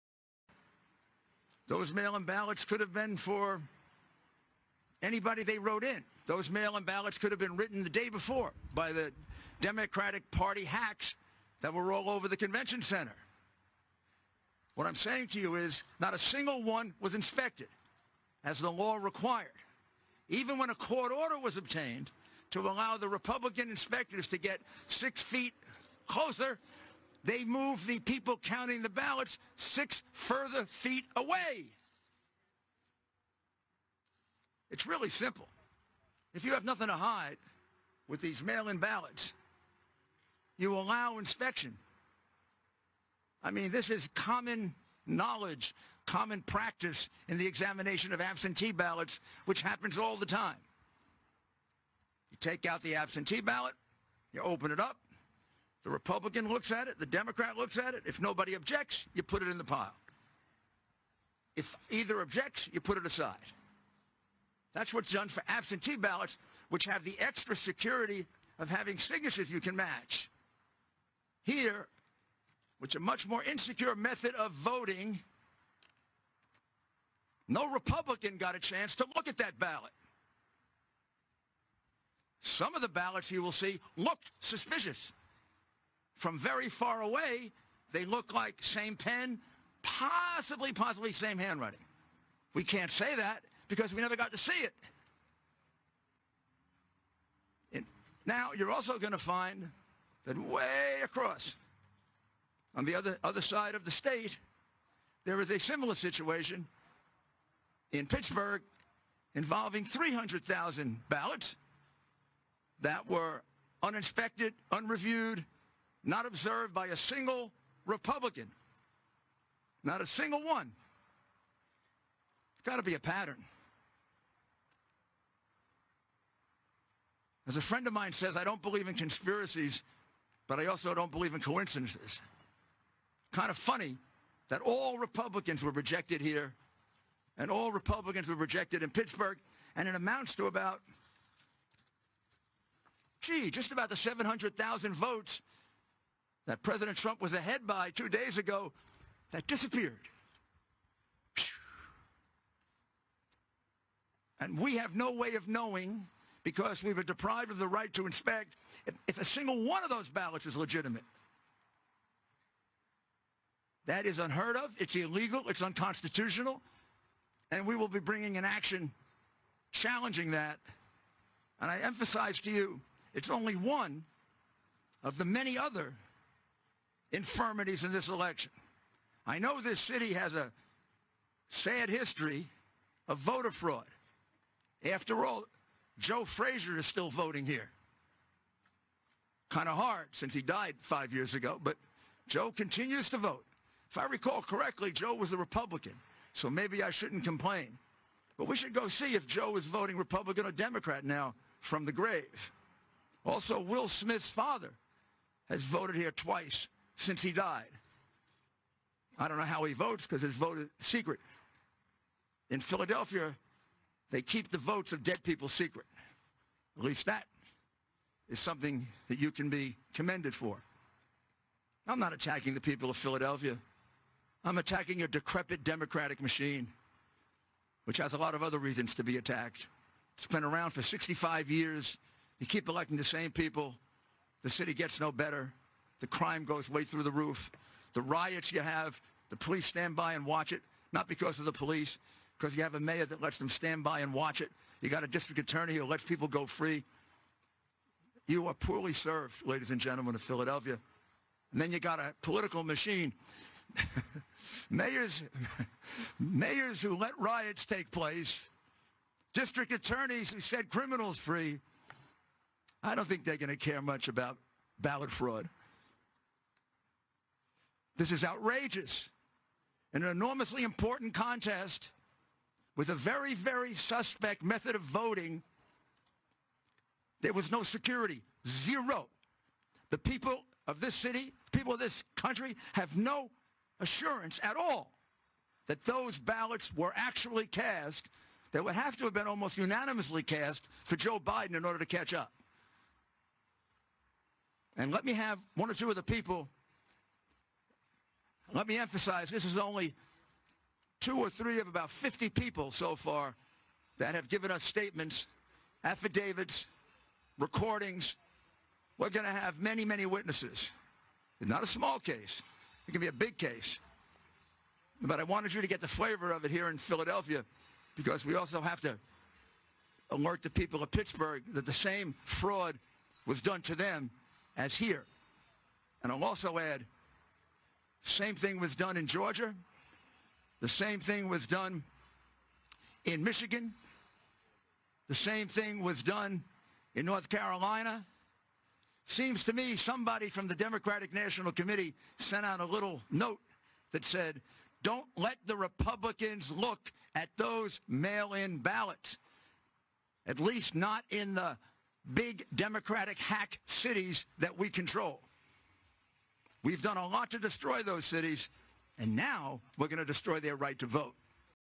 El equipo legal de Trump responde a los medios que falsamente declararon al corrupto Biden ganador.
11/7/2020. FILADELFIA: Rudy Giuliani dirige una conferencia de prensa sobre las acusaciones de fraude electoral inmediatamente después de que las principales cadenas de noticias convocaron la carrera presidencial a favor del candidato demócrata Joe Biden.

Noticias en blanco y negro dice: Es por eso que Biden no tuvo rallys de campaña, ¡su plan era robar las elecciones! No tuvo que hacer campaña.

La falta de transparencia es muy preocupante y demuestra además que el partido demócrata es el partido de la corrupción.

Estados Unidos es ahora es un país del tercer mundo, corrupción masiva.

¡Todo Estados Unidos sabe muy bien que el presidente Trump ganó en landslide!